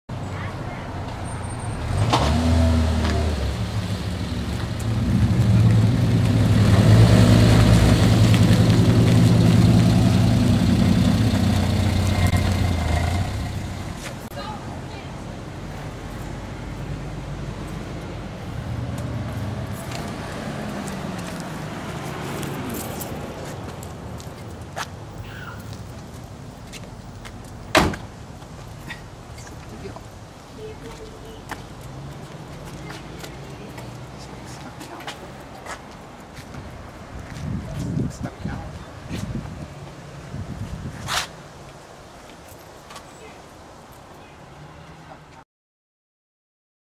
NiceX19.mpg
Bernice brings home a 1974 Fiat X1/9.